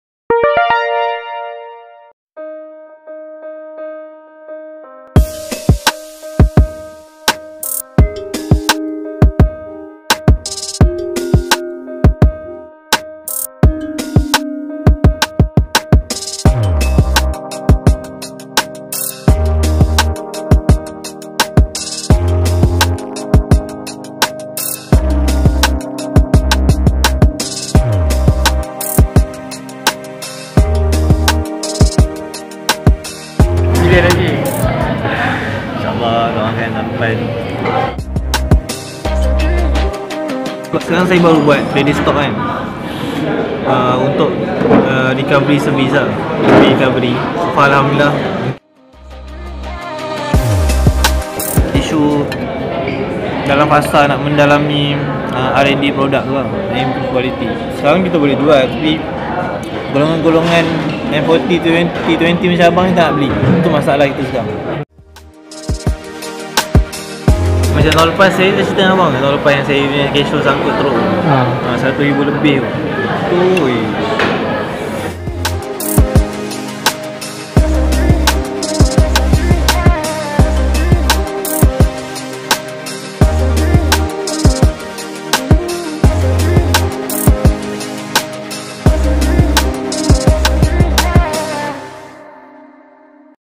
vlog #108 - Sedetik bersama FAHMI ABD RAHMAN
Pertama kali kami bertemu adalah ketika mengikuti program diploma keusahawanan yang dikendalikan oleh Abang Abu.